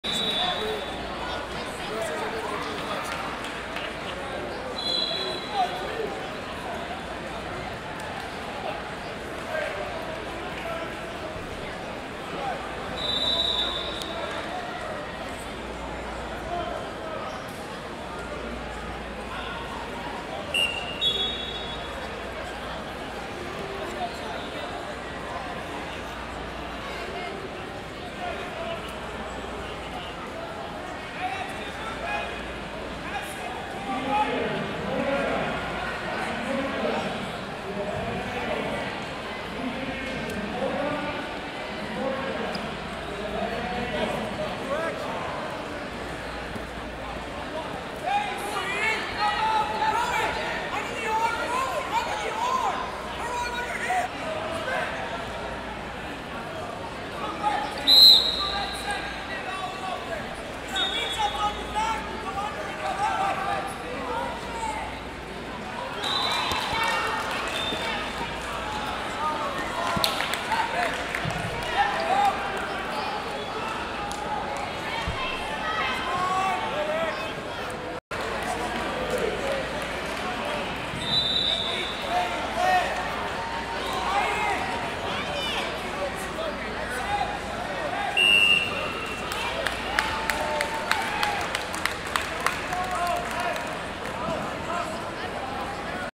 125 Lbs Round 1 (8 Team) - Taina Fernandez, Maryland Vs Kaden Hensley, West Virginia 788d
Flosports-partner-access/2023 14U National Duals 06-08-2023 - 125 Lbs Round 1 (8 Team) - Taina Fernandez, Maryland Vs Kaden Hensley, West Virginia 788d